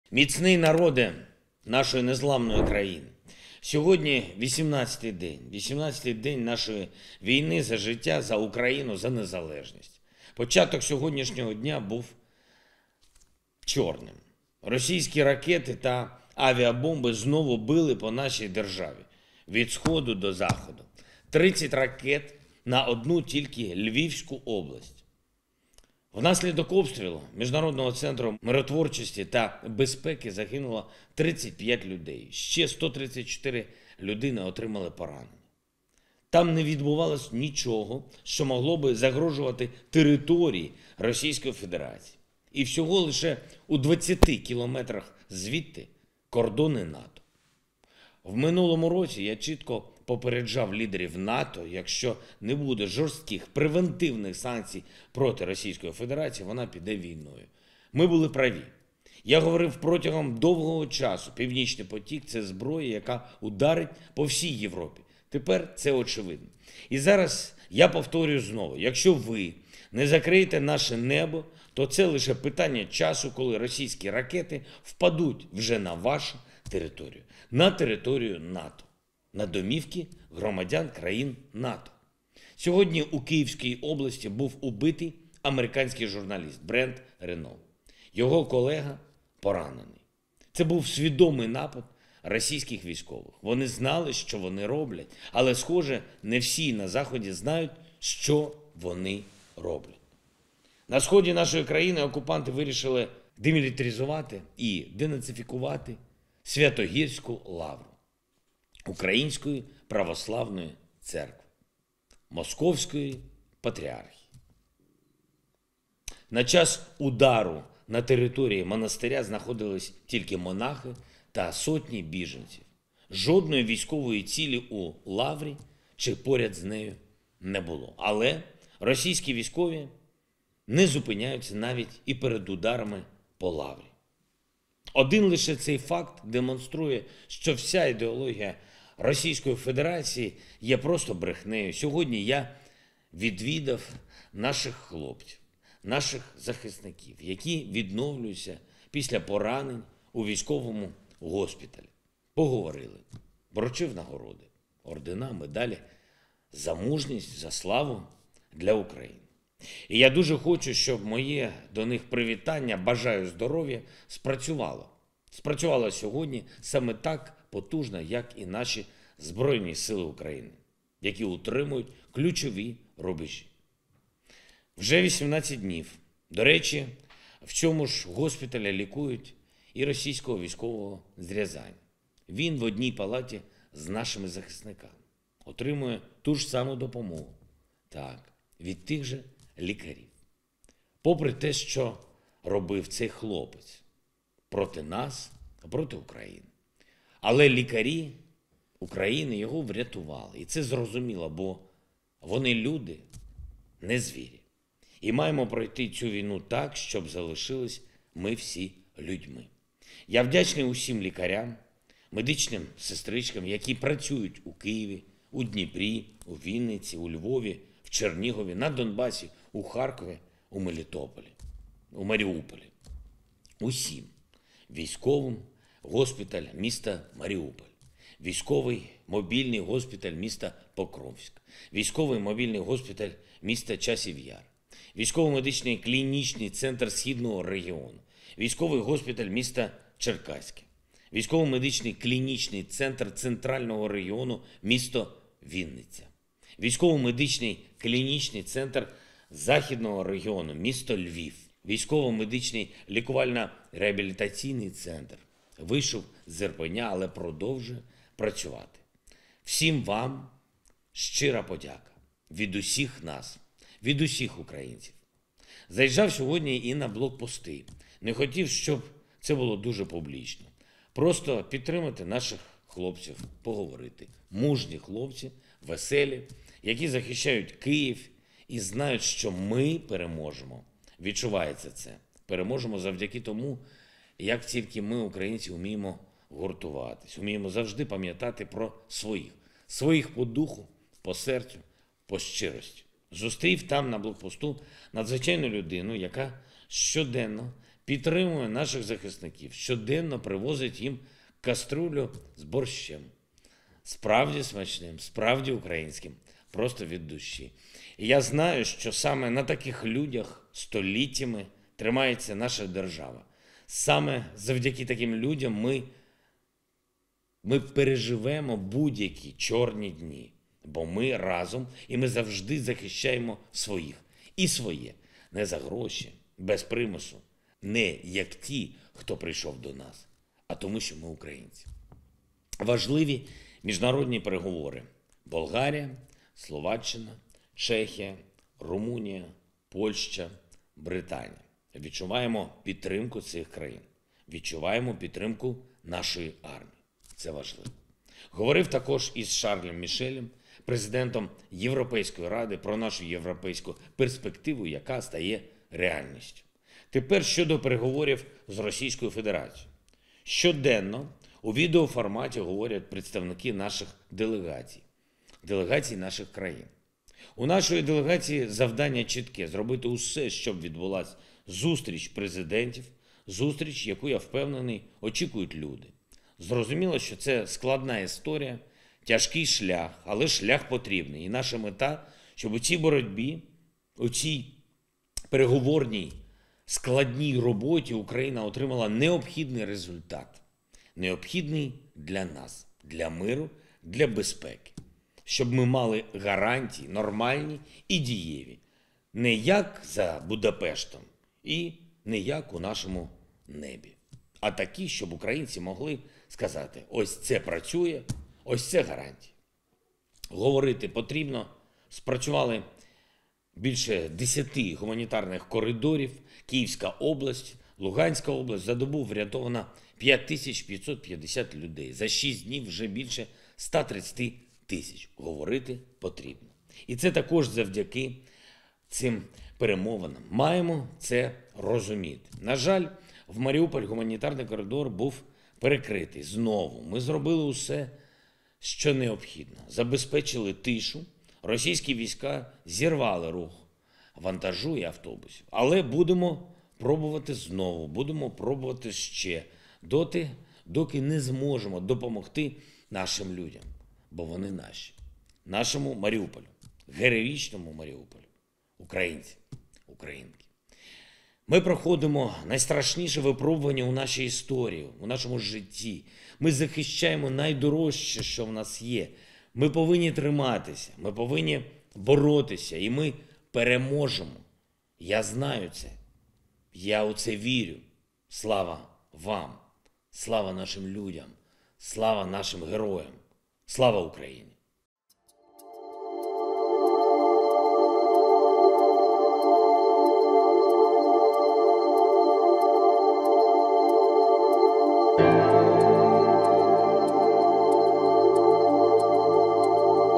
🔥ЗЕЛЕНСЬКИЙ: Ми проходимо найстрашніше випробування у нашій історії. Але ми переможемо - Україна 24
"Ми проходимо найстрашніше випробування у нашій історії, у нашому житті. Ми захищаємо найдорожче, що в нас є. Ми повинні триматися, повинні боротися. І ми переможемо, я в це вірю", - наголосив глава держави у своєму зверненні.